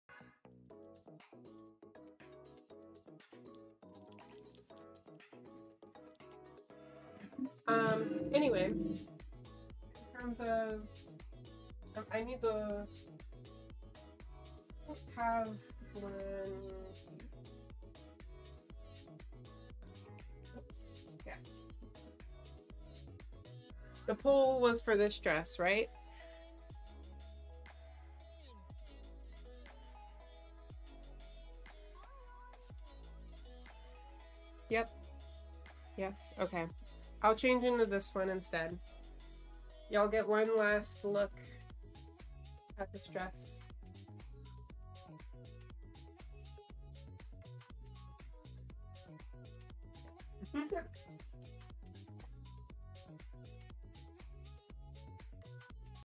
Last look at the dress with the weird strap thingy
This is a clip from Peachy's Twitch stream entitled HELP ME PICK A OUTFIT!

its_peachybunnn retains all rights to the stream content except where claimed by legitimate IP rights-holders.